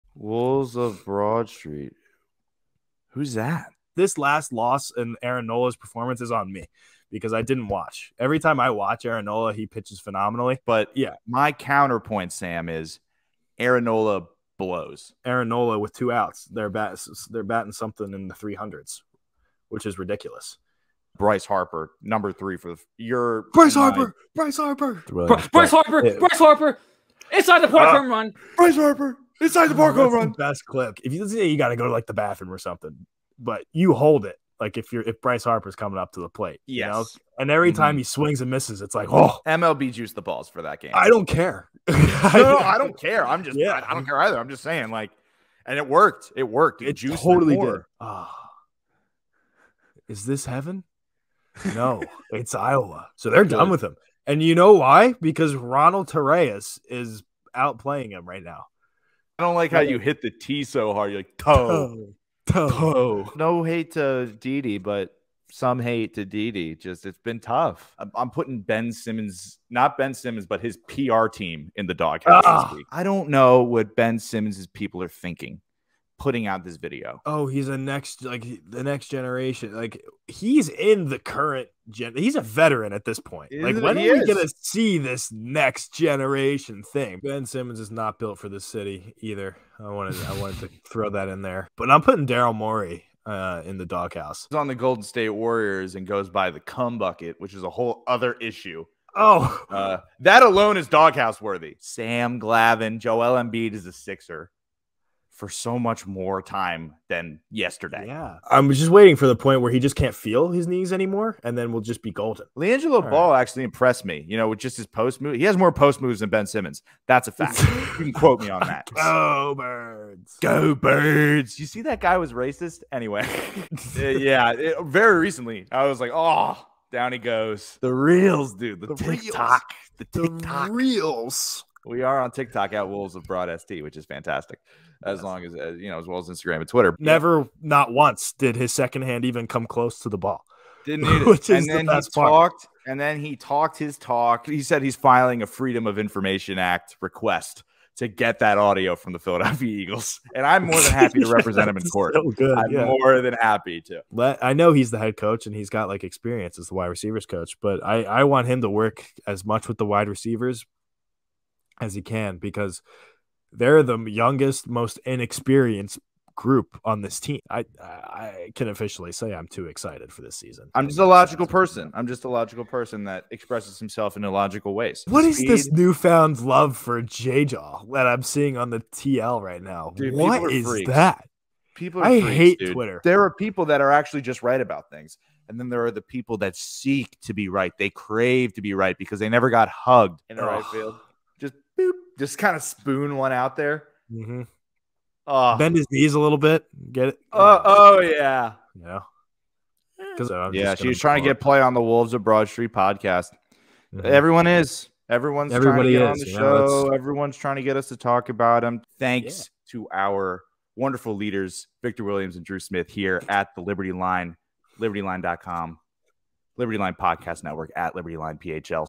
Episode 66: Bryce Harper! Bryce Harper! (Highlights)
Highlights from episode 66 where the Wolves zero in on the Phillies' strengths and weaknesses, talk about the Sixers resigning Joel Embiid, and praise the Eagles' young receiving corps (and head coach!) Plus, MLB finally did something right, Ben Simmons' PR people are brain dead, and we HATE the NBA.